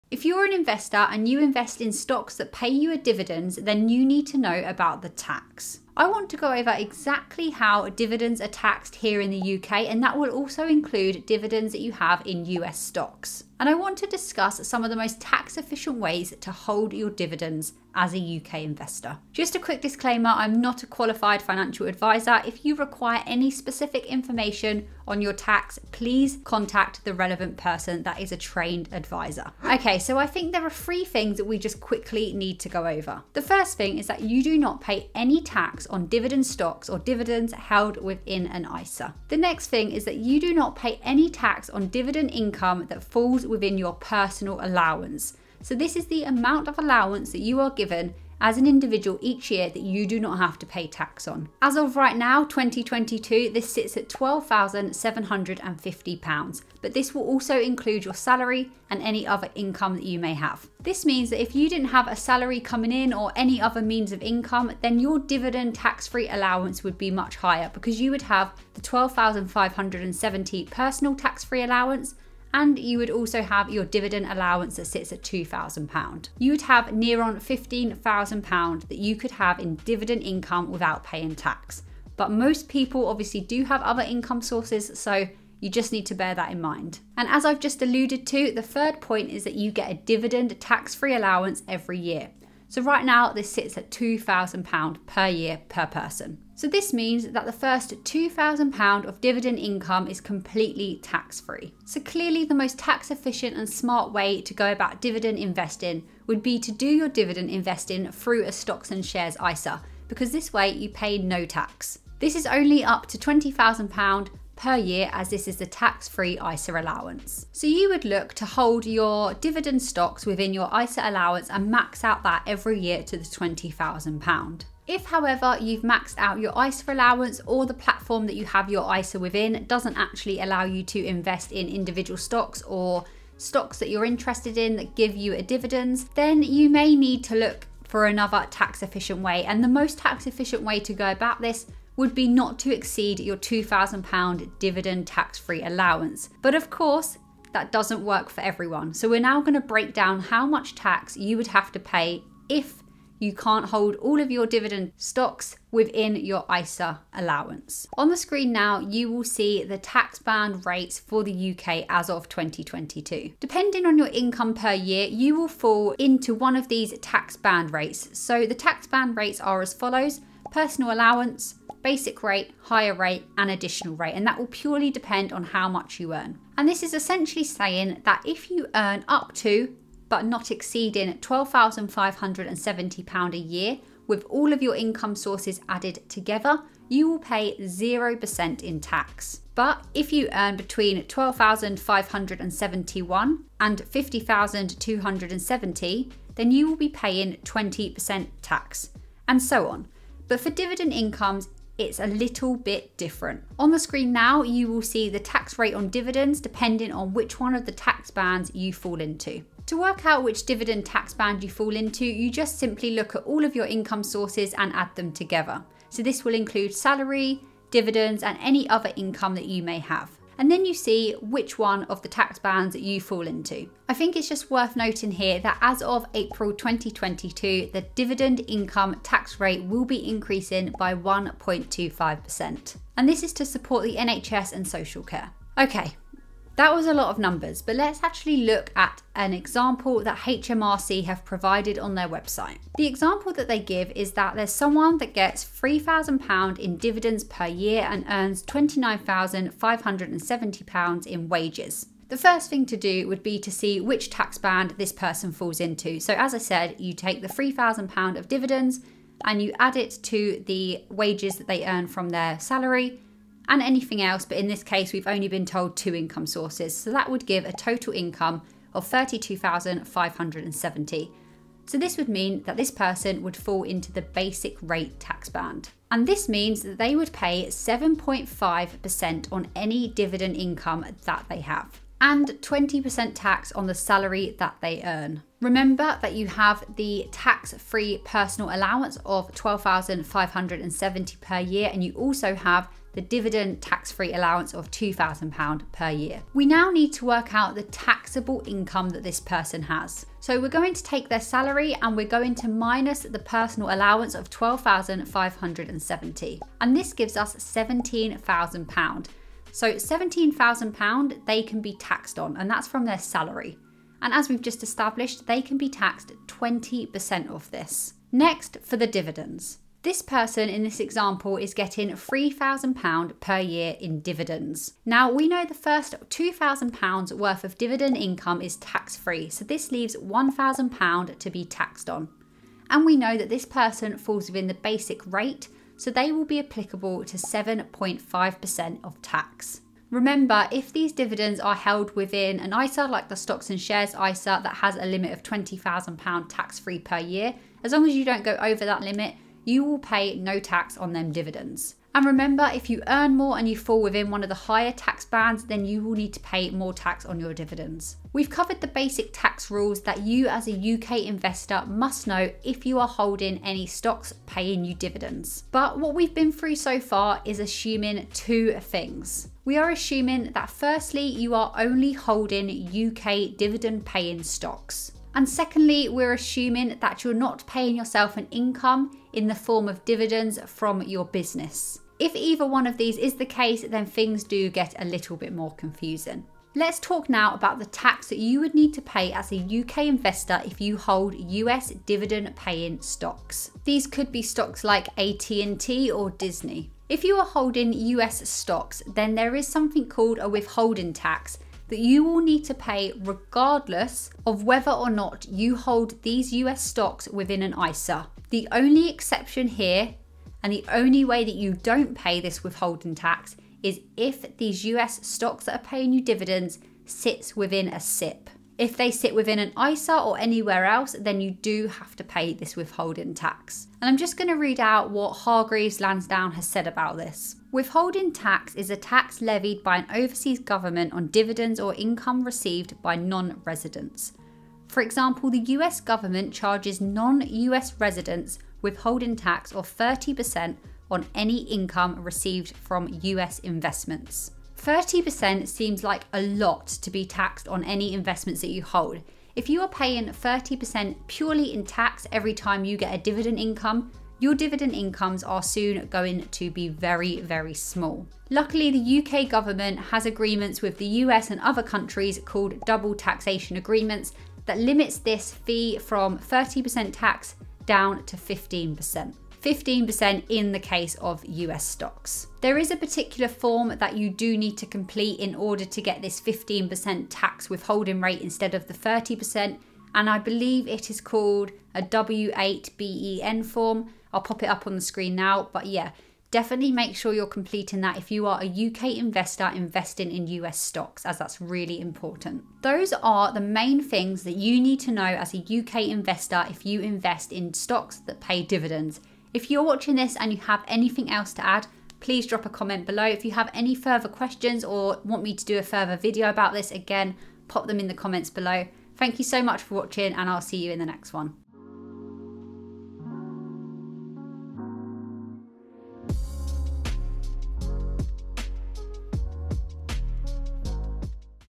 Dividend Tax U.K. | protect your dividends against high tax
Want to use a dividend portfolio to generate passive income? Well it's important to know how much tax you will need to pay on UK and US dividend stocks. I will discuss the best ways to legally pay as little tax as possible when investing in dividend stocks.

Flexible tripod

Books:
Rich Dad, Poor Dad - Robert Kiyosaki
How To Own The World - Andrew Craig

Products mentioned/seen/used to create video:
Rocketbook Core